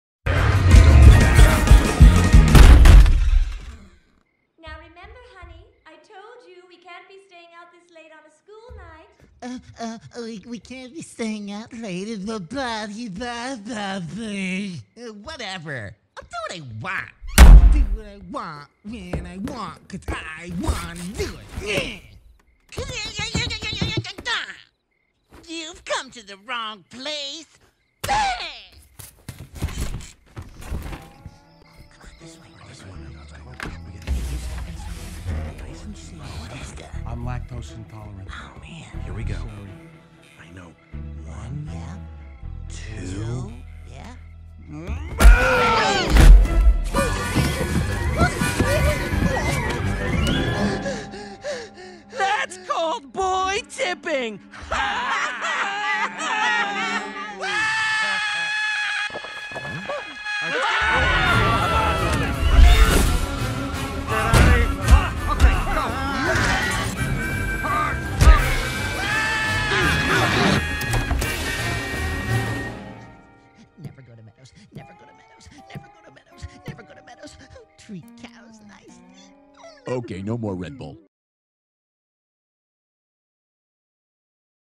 Baby Doll Tipping
Plot Summary
Baby Doll (Steve Oedekerk) gets what he deserves when the bat-heroes decide to push back.

Cast
Batman also voiced Otis - Kevin James
Batgirl also voiced Eddy - S. Scott Bullock
Robin also voiced Igg - Maurice LaMarche
Darla Dimple also voiced Snotty Boy and Darla's Father - Steve Oedekerk
And Darla's Mom - Jill Talley

Supervisor Animators (Animated by Adobe Flash Professional CS6)
Batman, Batgirl and Robin - Robby Patterson (instead the animators of Michael Helmuth, Jesper Möller and Christian Kuntz)
Darla Dimple - Jackson Patterson (instead the animator of Jesper Möller)
Darla's Father and Darla's Mom - Johnny Patterson (instead the animator of Christian Kuntz)